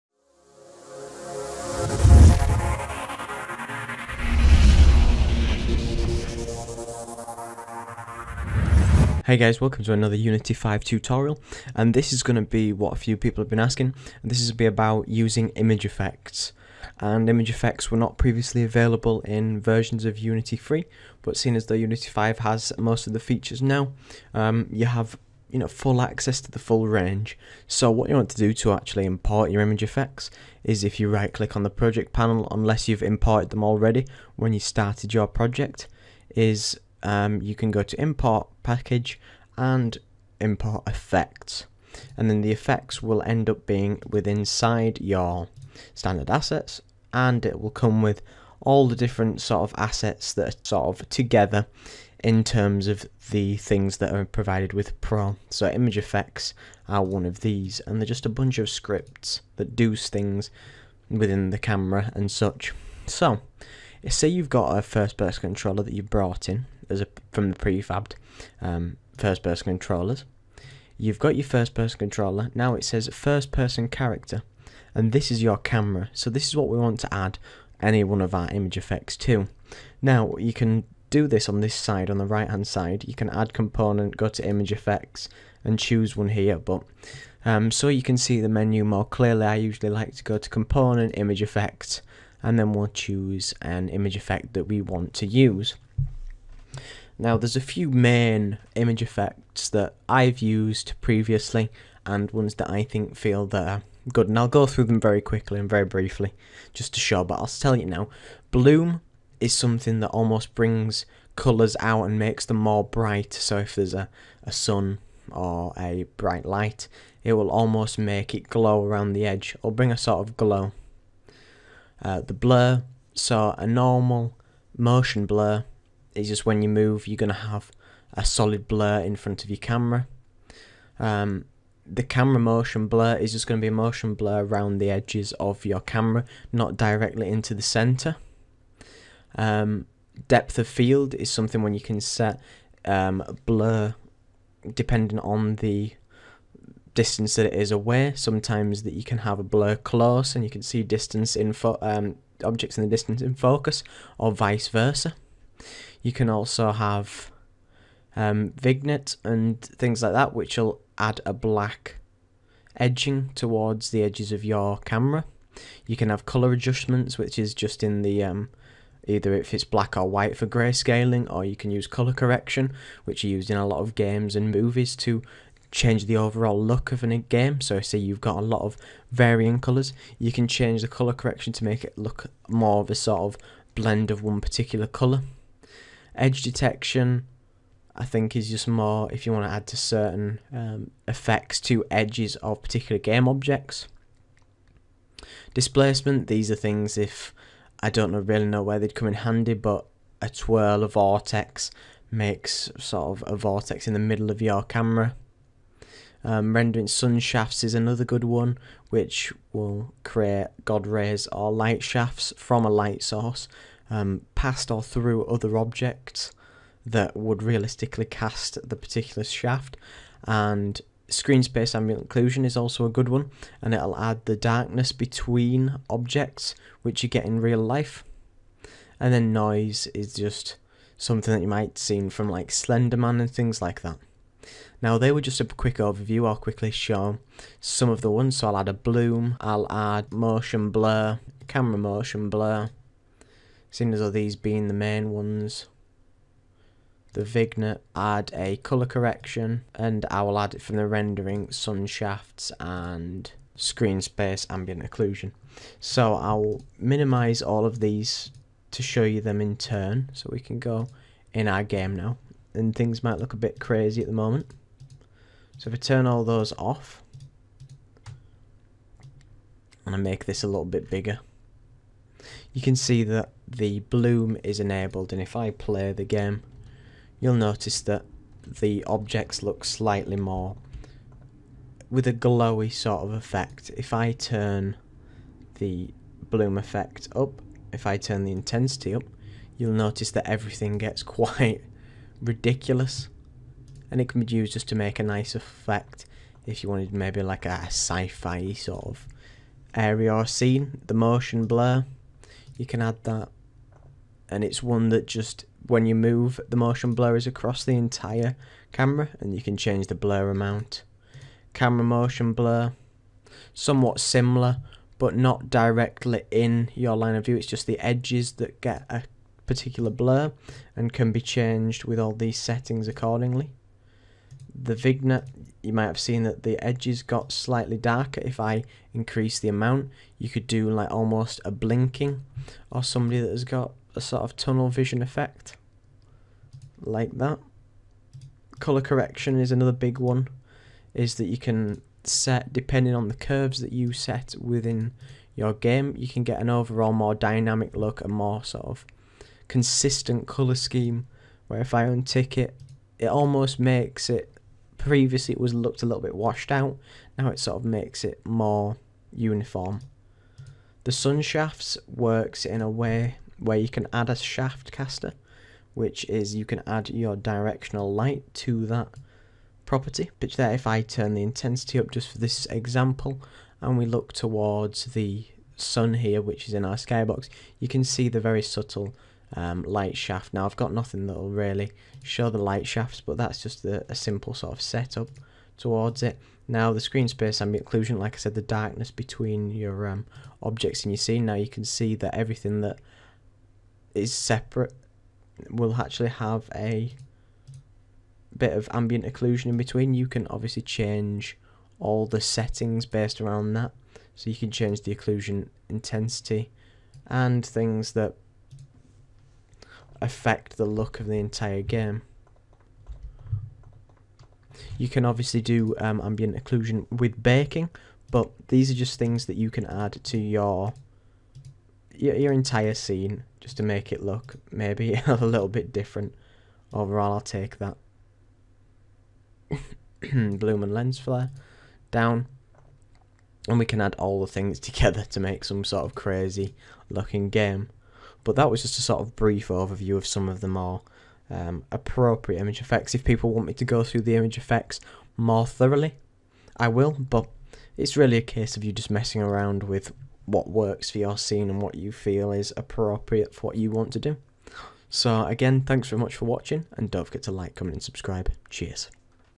Using Image Effects in Unity 5
I show you how to use image effects in Unity 5, including: Sun shafts, Vignetting, blur, colour correction, bloom and more! This will briefly show you the setup and use of common image effects for use in your Unity 5 games!